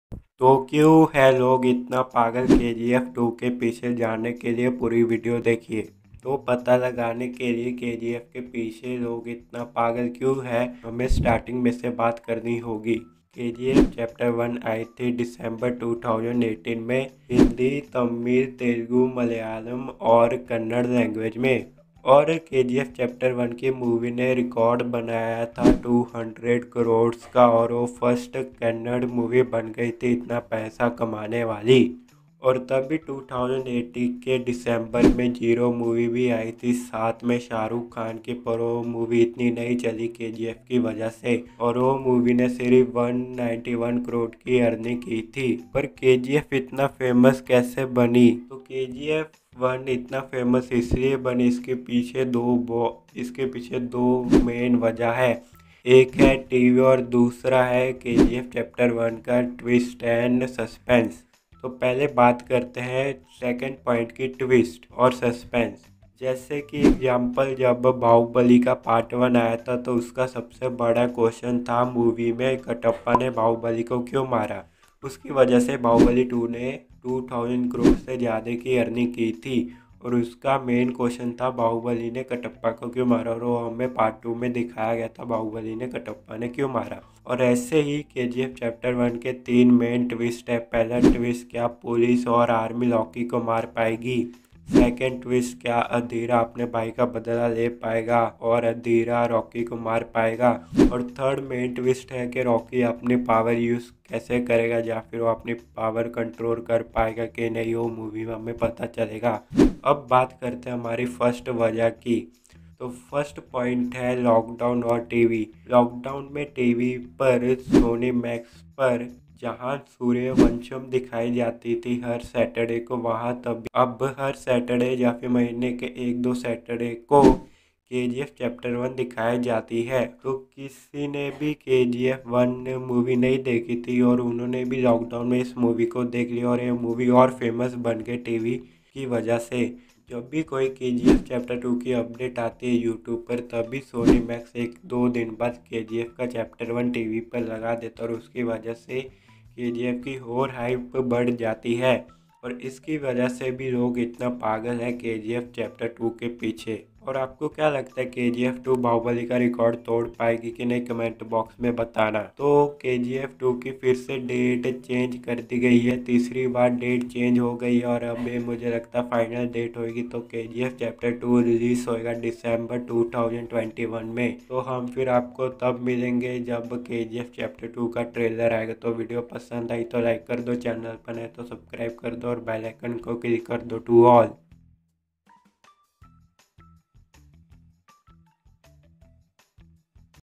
KGF Review - Kya Aa Monster Bahubali Ko Beat Kra Pagyi?
Today We Reviewing, Recaping KGF Chapter 1 For  Knowing That Watch This In The Video. Drop A Like On This Video. Star Cast Rocking Star Yash & Sanjay Dutt

Ignore (Tags):-
Kgf review
kgf movie review
Kgf chapter 2 review